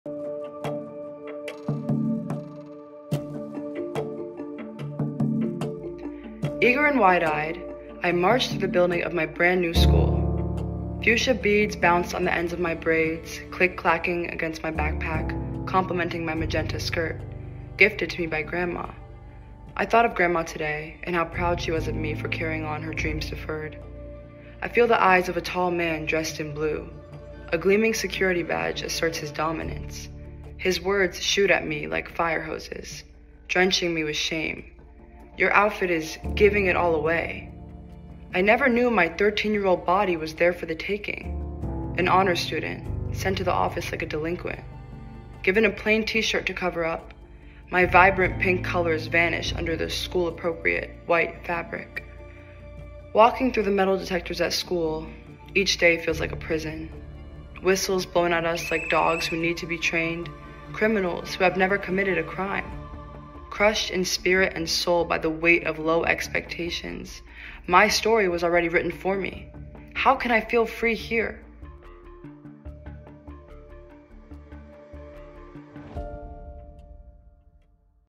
“I Can’t Breathe” Animated Video Release
This animated video brings to life the poignant poem, "I Can't Breathe" by Makayla Rivera, our esteemed Youth Storyteller in Residence. The animation serves both as a medium for Makayla’s powerful voice and also as a companion piece for our latest report, "Fulfilling the Promise: A Blueprint to Build Police-Free Schools." This report outlines actionable steps toward creating safer, more inclusive educational environments for all students.

By watching and sharing this video, you help us spread awareness and foster a deeper understanding of the challenges and opportunities in creating safer, more inclusive school environments for all students.